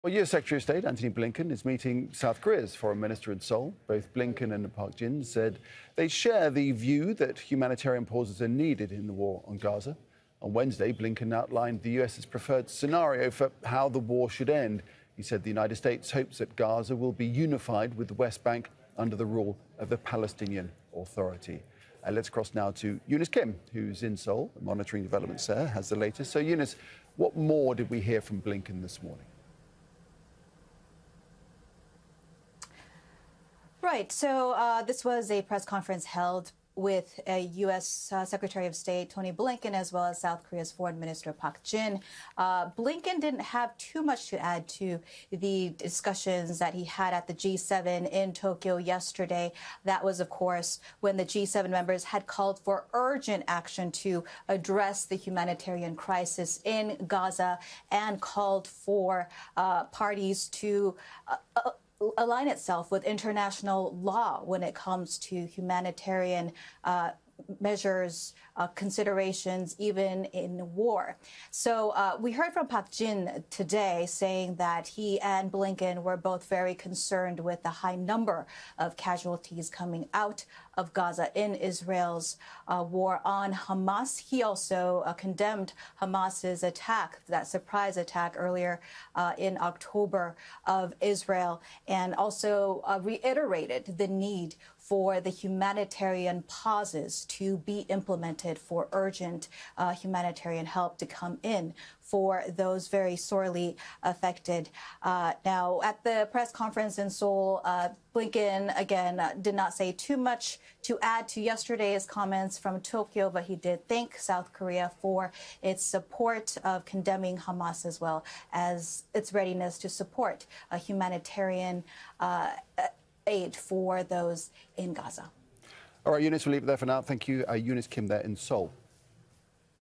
Blinken and South Korean Foreign Minister Park Jin call for humanitarian pauses in Gaza
US Secretary of State Antony Blinken is meeting South Korea's foreign minister in Seoul.
Both Blinken and Park Jin said they share the view that humanitarian pauses are needed in the war on Gaza.
On Wednesday, Blinken outlined the US's preferred scenario for how the war should end.
He said the US hopes Gaza be unified with the West Bank under the rule of the Palestinian Authority.

Al Jazeera’s Eunice Kim is live in Seoul with the latest.